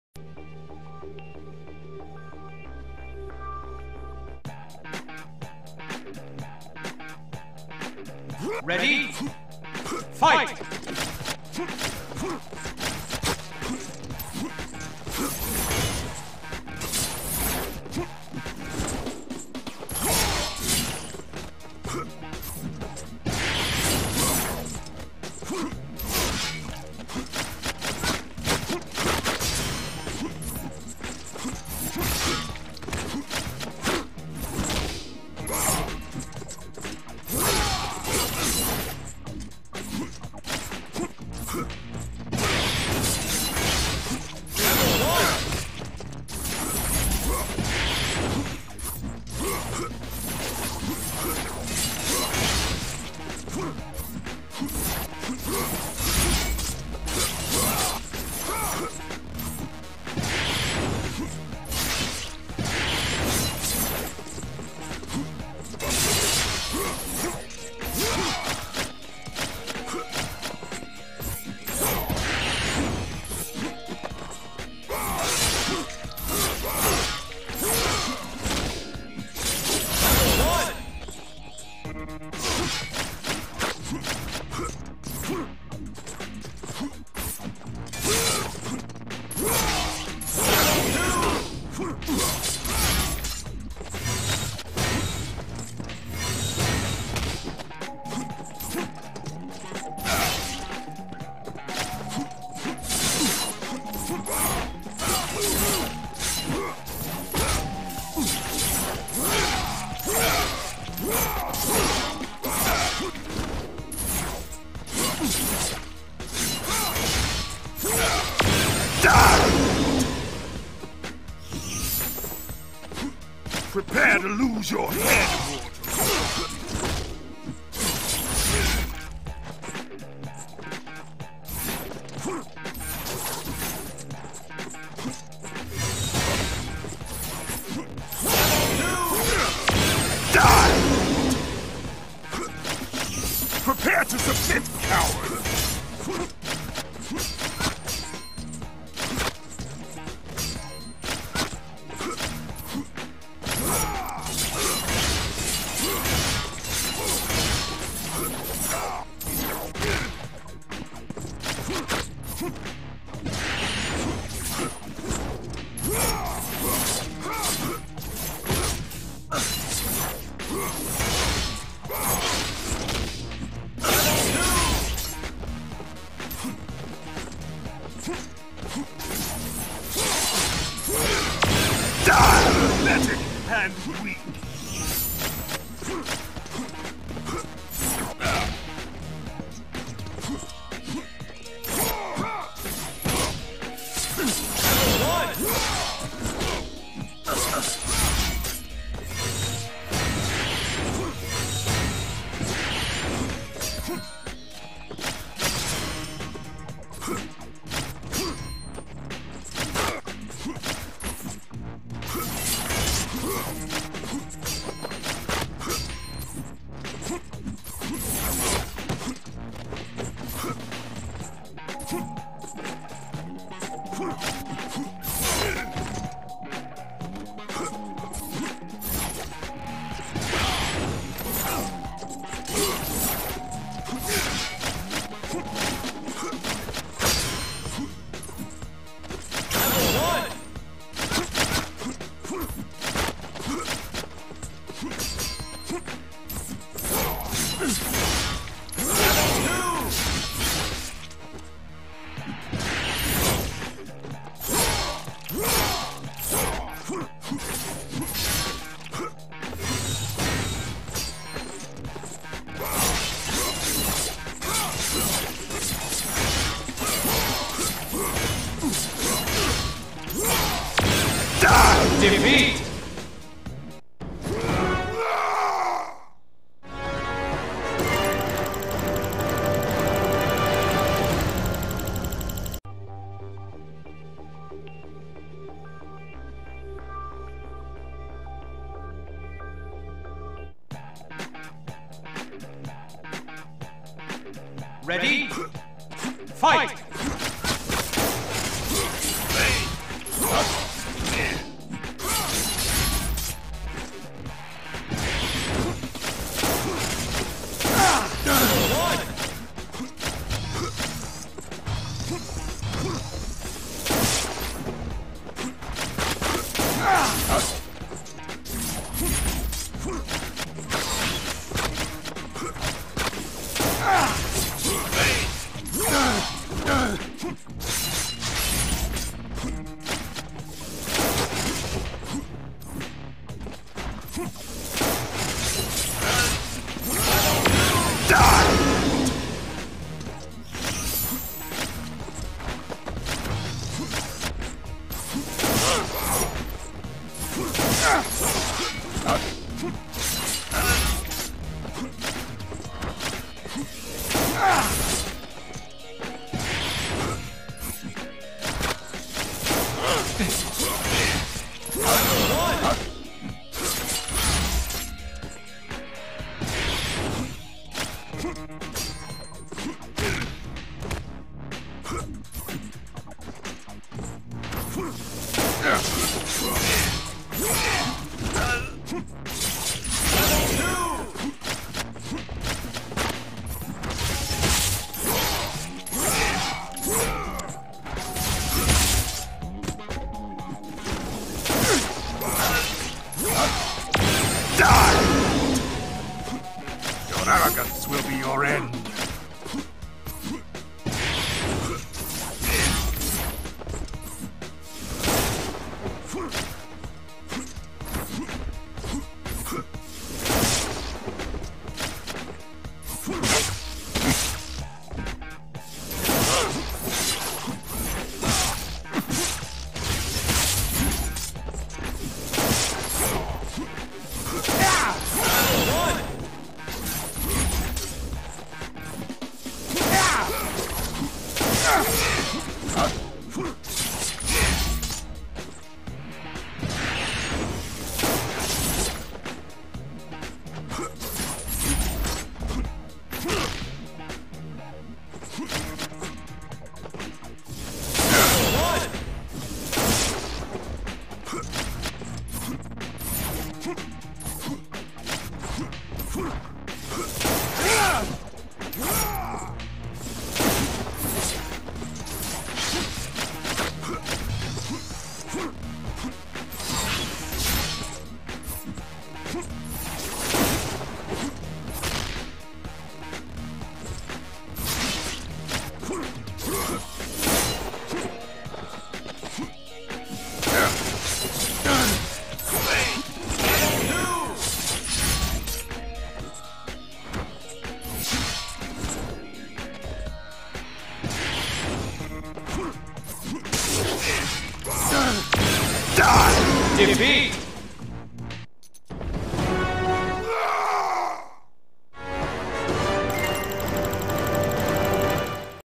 1v1 Exhibition: LIQUID12A VS I-Use-A-Blender(Curb-Stomp Battle)
Kratos beats most people(Captain Obvious) in this game. Even other Kratoses. Mine sucks, honestly. I wanted to see how fast I would lose.

BalanceOutTheLow-Tiers